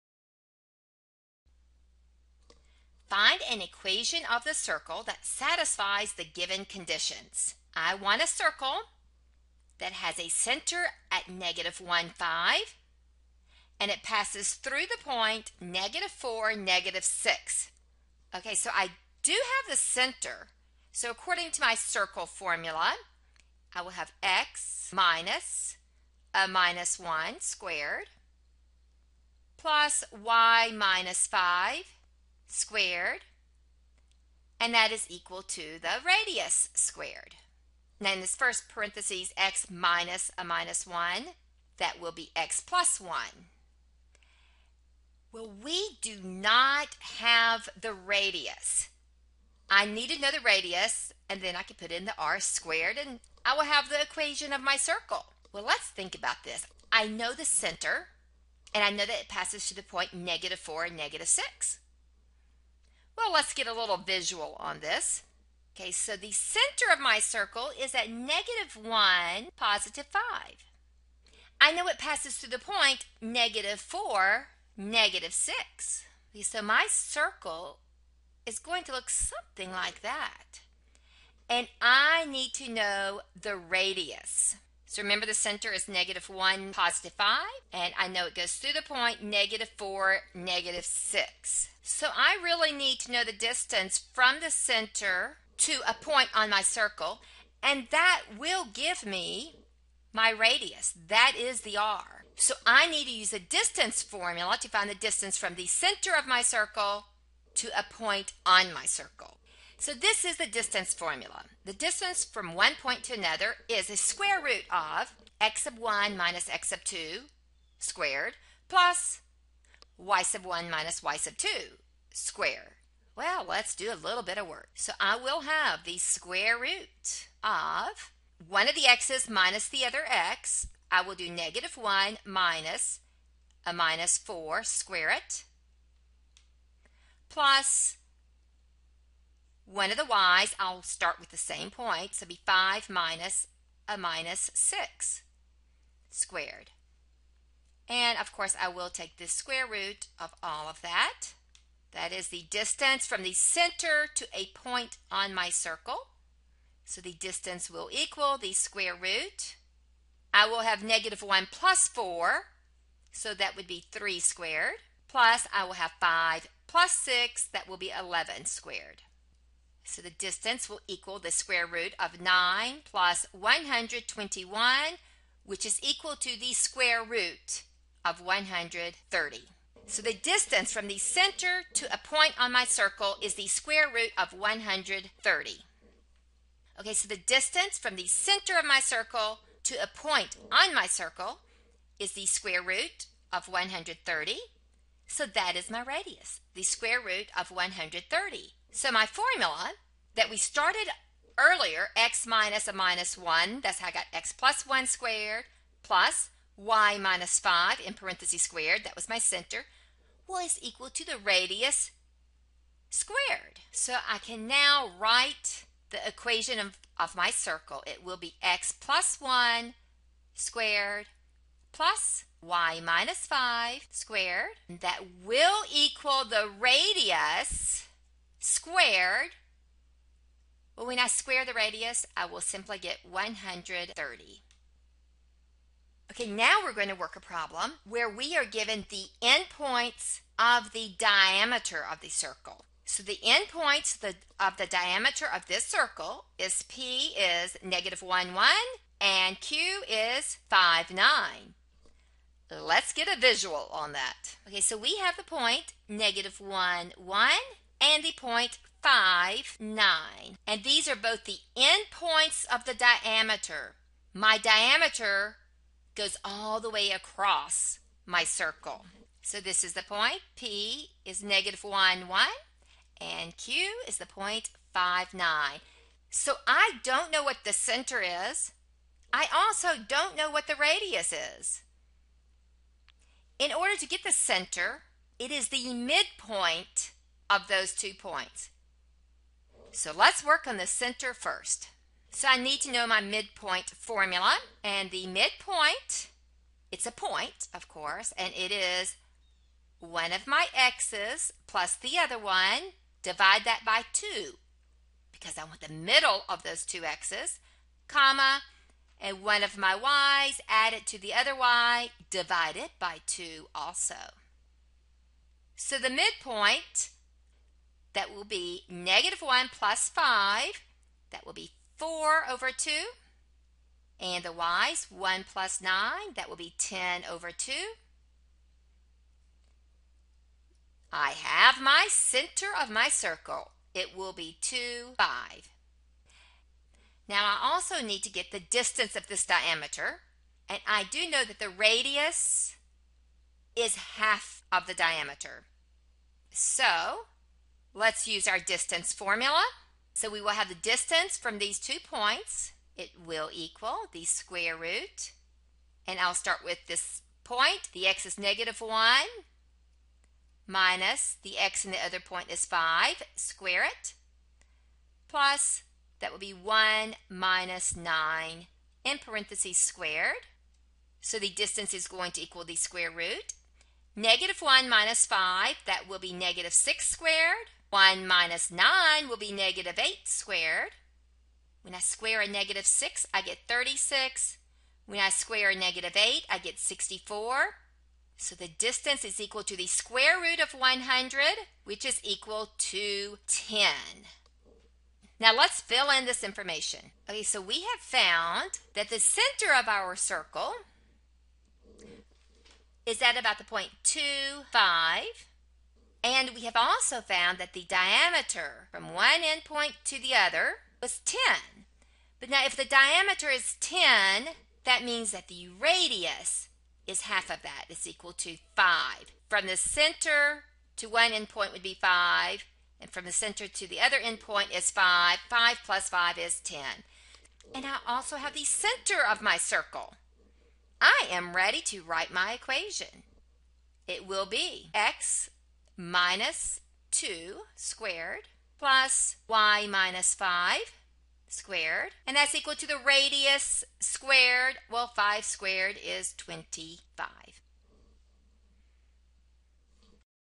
find the equation of a circle from the given information
We will find the equation of a circle given the center and endpoint of a circle and we will find the equation of a circle given the endpoints of the diameter